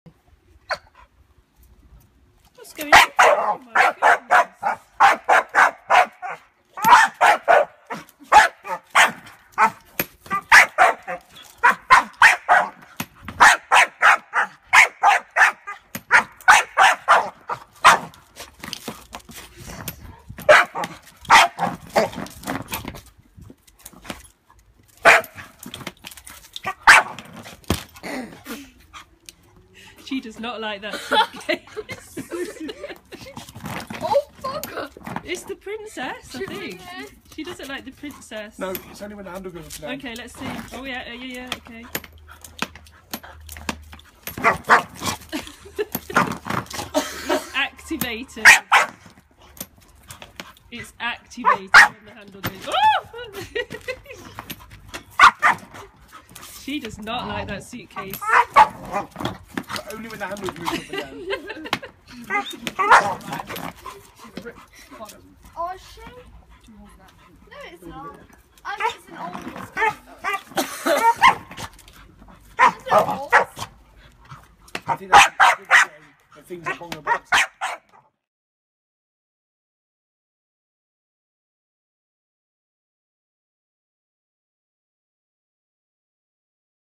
Random things that make Bongo Dog Bark: no 2, a suitcase. Dog barks at suitcase!
This is the second in a series of videos about the random things that make my dog bark, this time her reaction to my stepdaughter's suitcase. Bongo is a staffy x rottie and she's wonderful - but she barks at some really random stuff. She's not keen on horticulture, or The Quilt Monster. In this video, we see just how angry she can get at a suitcase. Especially if you move the adjustable handle up and down. She does not like that at all! My stepdaughter tests what's going on by moving the handle, and Bongo barks louder and louder, and attacks the suitcase. She really doesn't like the suitcase at all!

Bongo is a sweet lovely dog who wouldn't harm a fly; you wouldn't know it when you hear her around this suitcase, or garden implements. She's a very funny dog.